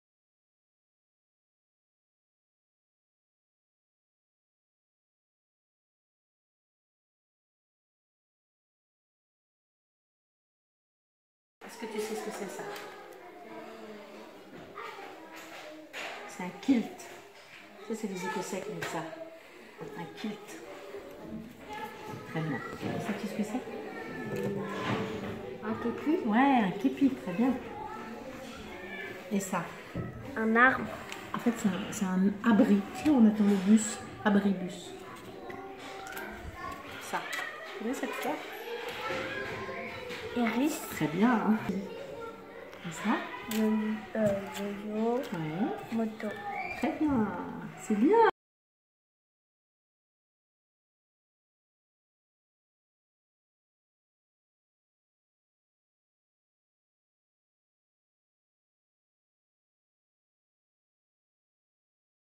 La méthode Montessori avec Sylvie d'Esclaibes : Lire des mots de 4 lettres
Dans la méthode Montessori, l'apprentissage se fait progressivement. Maintenant que votre enfant a vu des mots de 2 ou 3 lettres, apprenez-lui à lire des mots plus longs !